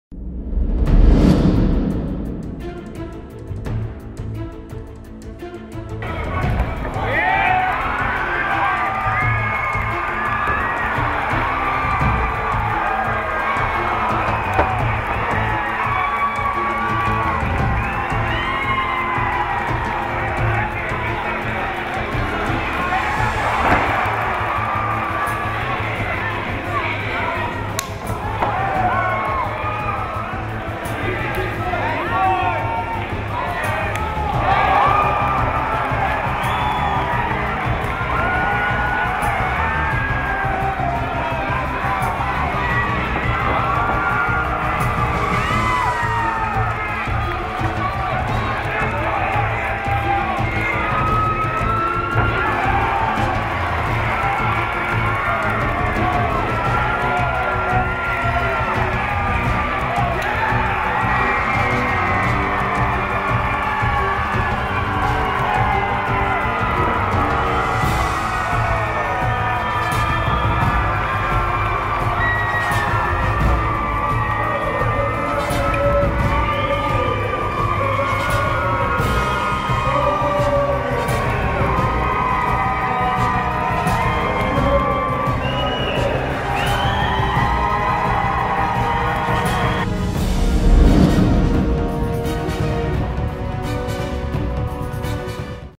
(HIGHLIGHTS) ToroMMAFighter: JASON SOARES v DANNY CHAVEZ
Check out the HIGHLIGHTS of one of our ToroMMAFighters JASON SOARES v his opponent DANNY CHAVEZ.